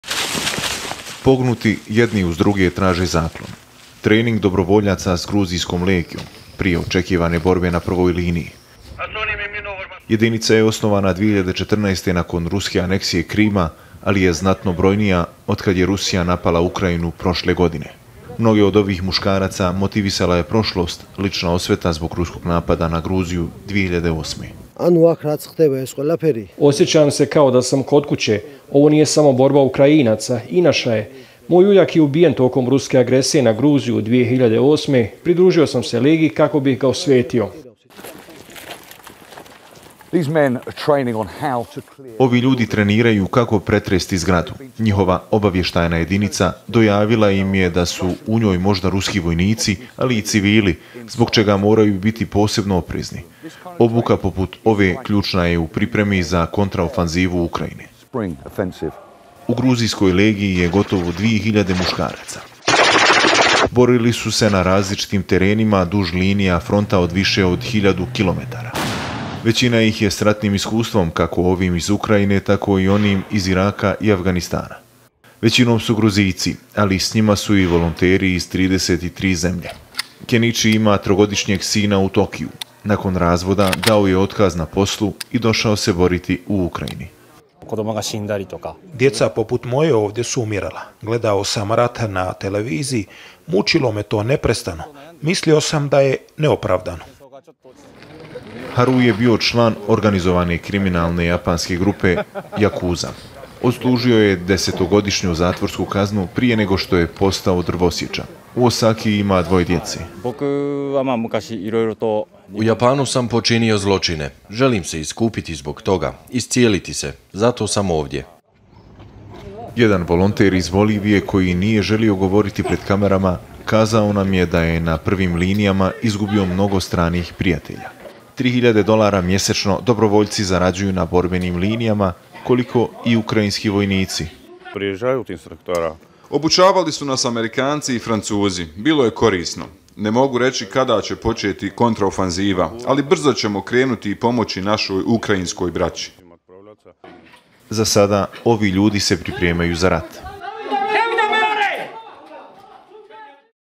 Gruzijska legija bori se protiv Rusa u Ukrajini
Gotovo pet hiljada stranaca bori se u Ukrajini protiv ruske vojske. Među njima su dobrovoljci iz Gruzije, koji se još sjećaju ruskih napada na njihovu zemlju iz 2008. godine.